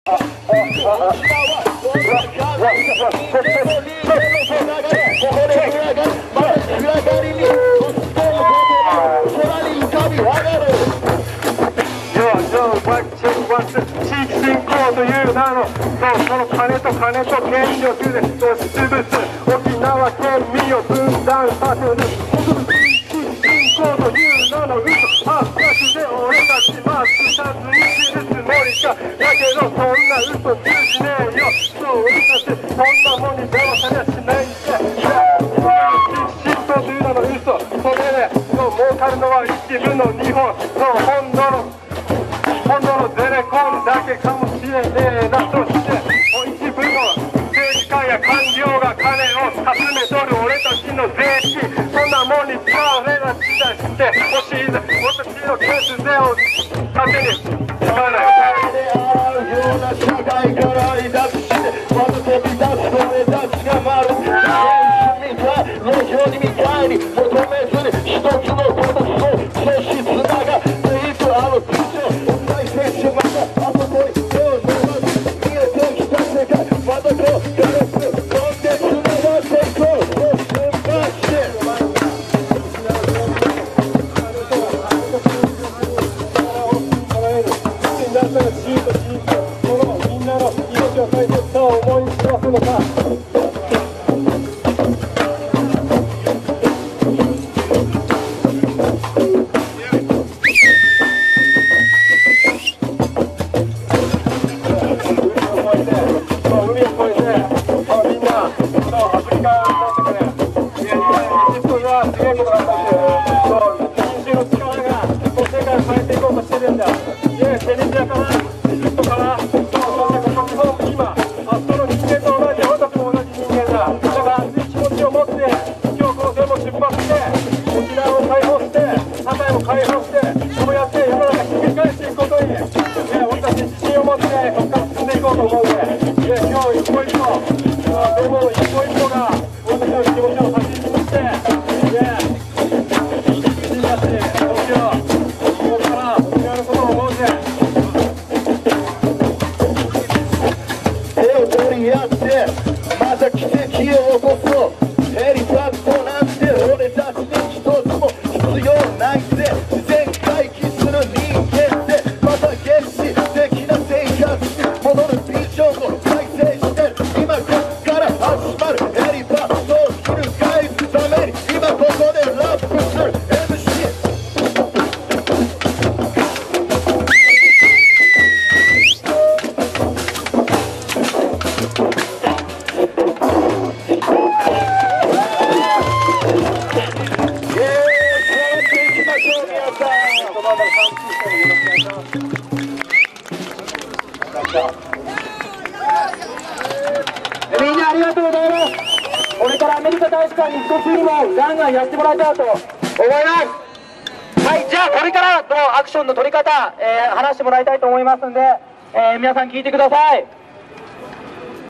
Takae Protest 2/20/2011: Japanese MC's Protest U.S. Military Helipad in Takae, Okinawa part 2/2
February 20, 2011.  Shinbashi, TOKYO - Protest against the construction of military helipads in Takae, Okinawa.  MC's = Yuzo and Ryo.  Sa-chan leads protestors on drums.  For more information on Takae, visit the following links: